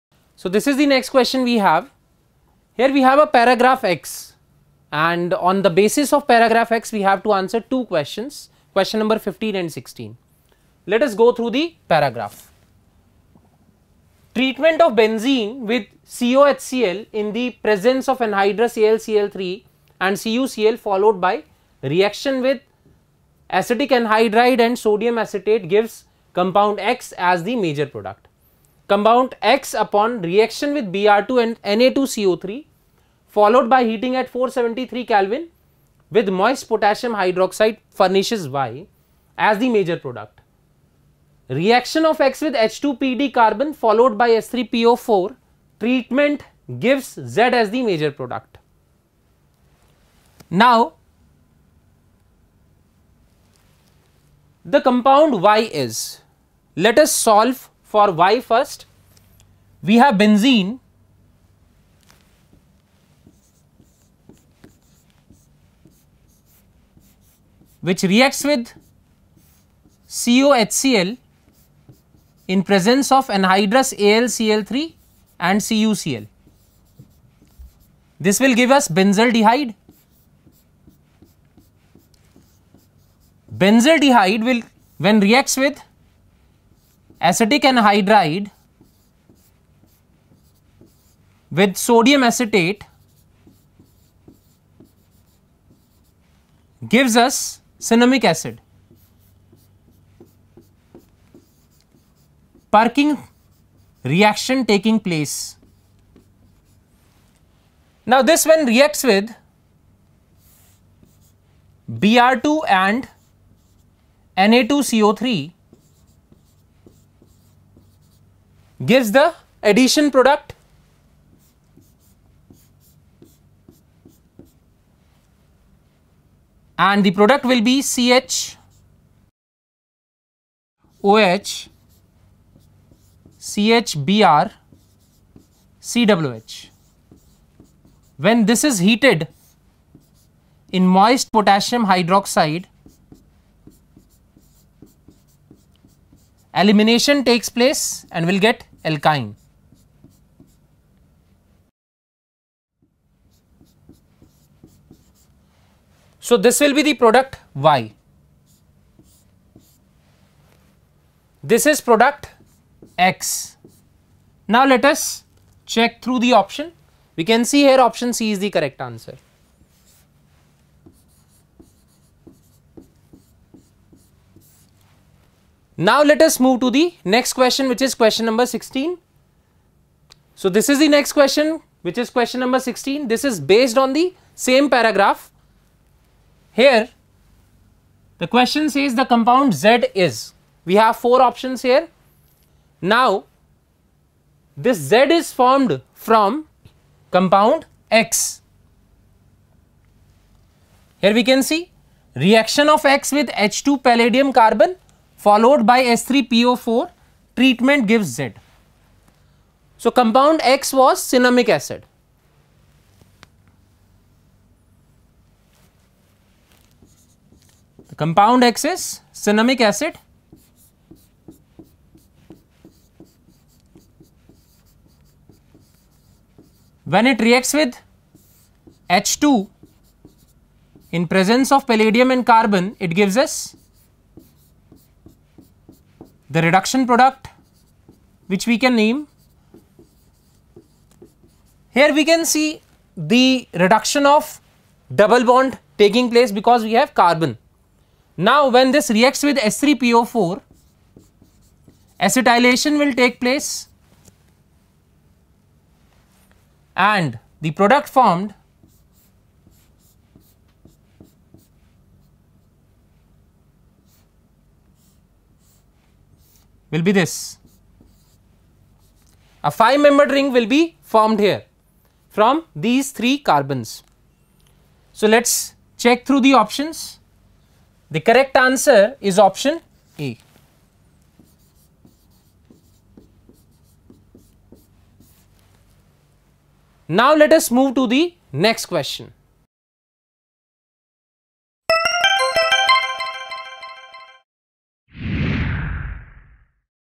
JEE Advanced Answer keys & Solutions 2018 for Chemistry (Paper-1) Q.15-16 | Aakash Institute
Find JEE Advanced 2018 Chemistry (Paper-1) answer keys and solutions for question 15-16, prepared by Aakash Institute. This video gives complete in-depth explanation to the questions. Know the solutions in detail with the help of subject experts. The experienced faculty at Aakash is committed to every student’s academic well - being and offers excellent guidance to crack entrance exams.